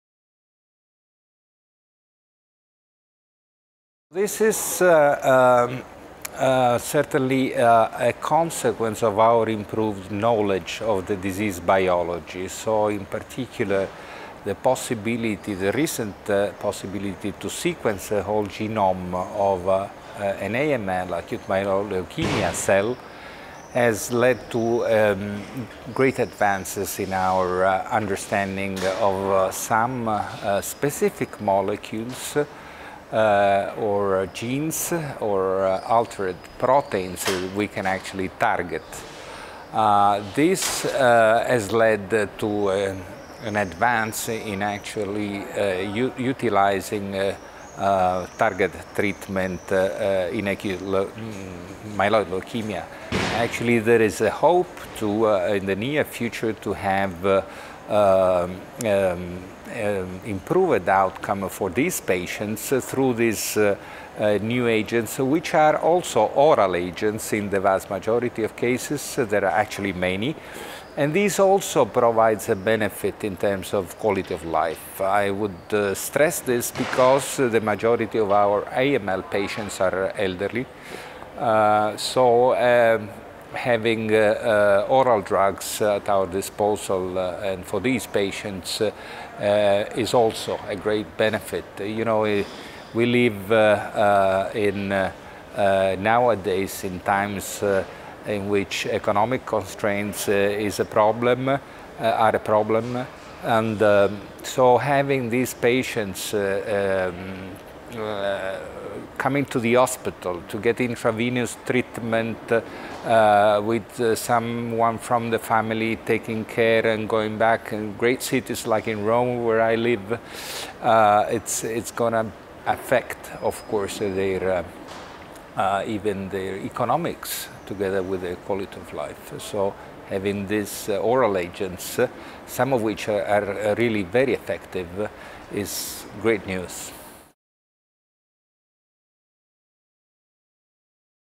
How genome sequencing can help improve quality of life in AML patients
Acute myeloid leukemia (AML) treatment can be particularly stressful for patients, many of whom are elderly. Francesco Lo-Coco, MD, from the University Tor Vergata Rome, Rome, Italy, talks to us about how recent advances in our ability to screen the genome of AML cells has led to the development of novel targeted treatments. In addition, Dr Lo-Coco explains the impact these new agents will have on the quality of life of patients undergoing treatment. This interview was filmed at the European Hematology Association (EHA) 2017 Annual Congress in Madrid, Spain.